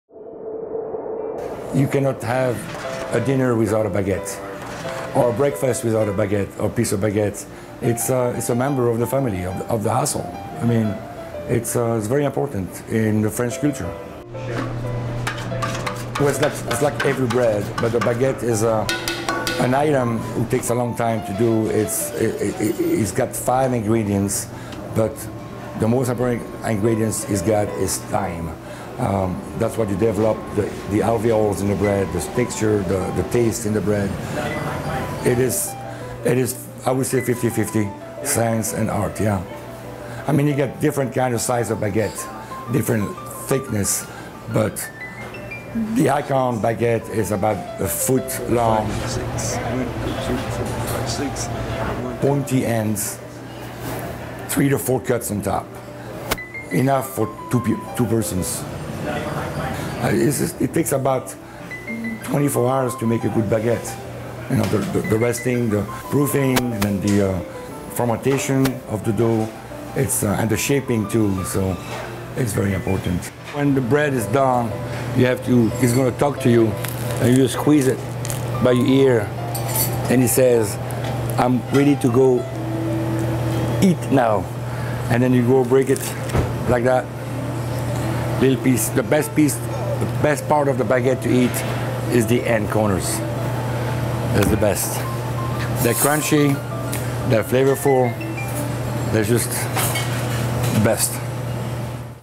Our Daily Bread: The French love their baguettes
In a daily ritual, Tadeusz Wielezynski and other bakers prepare baguettes at La Chatelaine.